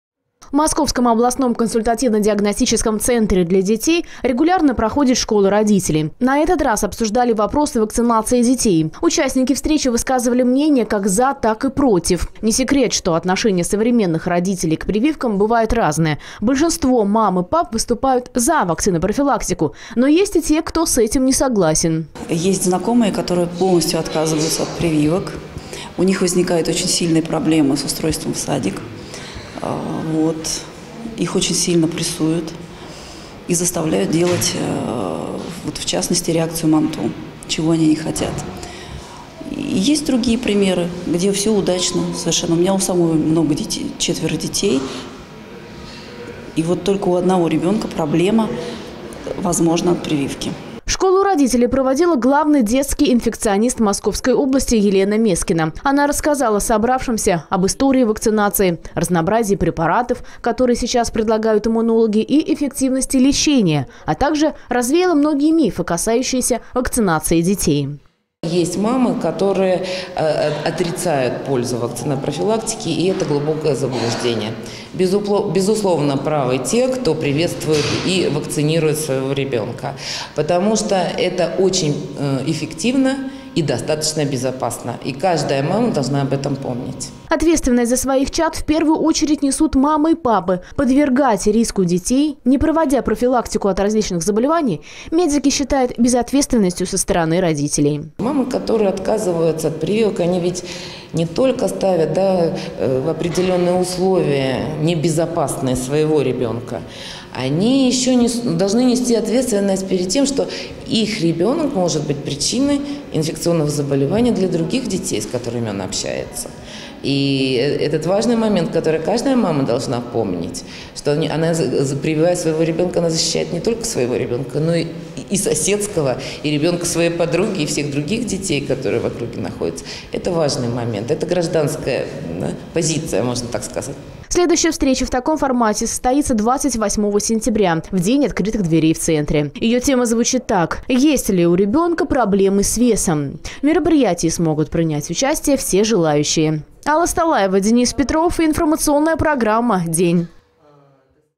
Вакцинация «За или против?»
«Вакцинация: за или против?» -этой теме был посвящен очередной семинар в  региональном консультативном центре для детей. Главный детский инфекционист Московской области Елена Мескина рассказала о правилах вакцинации и ее важности. Алла Столаева с подробностями.